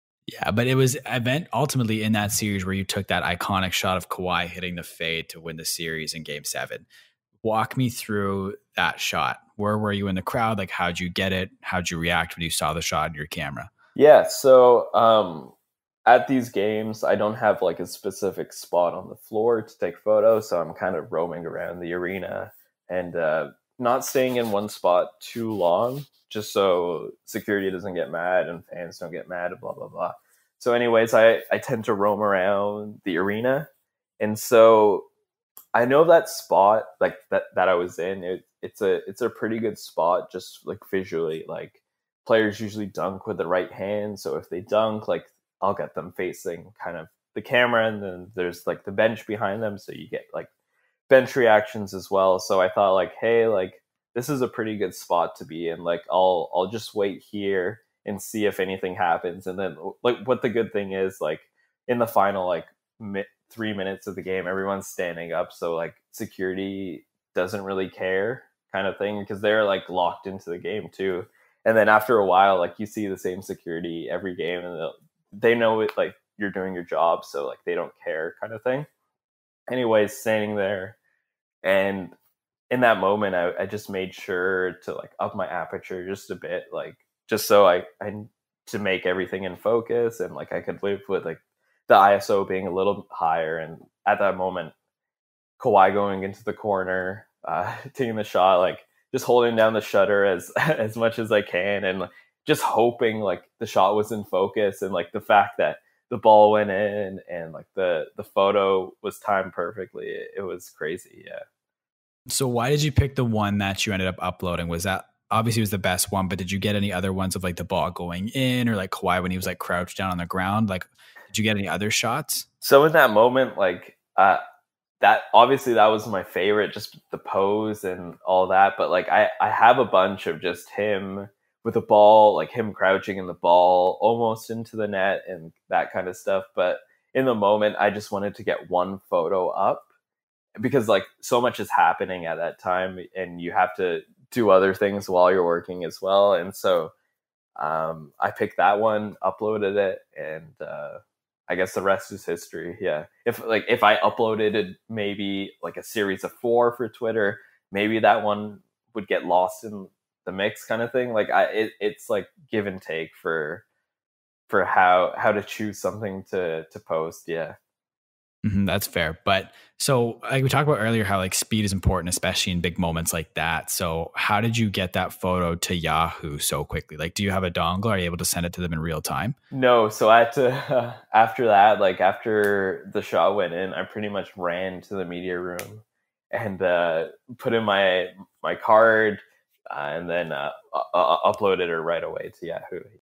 The Story Behind this Iconic Photo of Kawhi's Buzzer Beater
Kishan Mistry is a photographer for Yahoo Sports Canada and his assignment this spring was to document the Toronto Raptors playoff run. During that time he took this iconic photo of Kawhi sinking the first Game 7 buzzer beater in NBA history. Here's the story of how he got it.